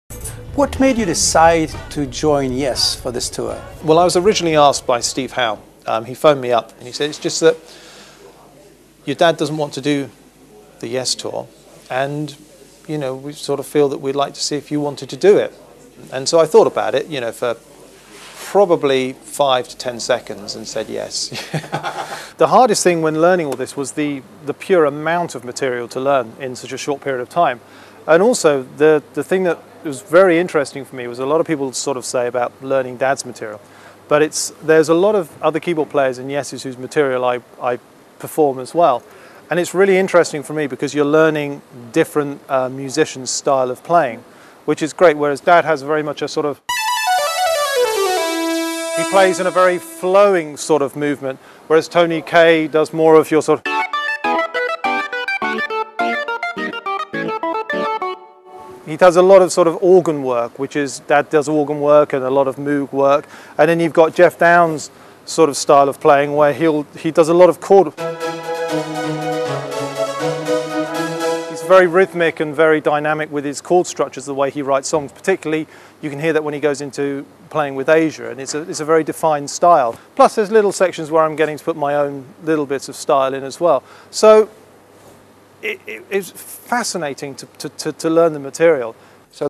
Yes Interviews: 2011 - Oliver Wakeman explains the different Yes keyboardists' styles
Yes Interviews
Date: 2011
Item: Oliver Wakeman explains the different Yes keyboardists' styles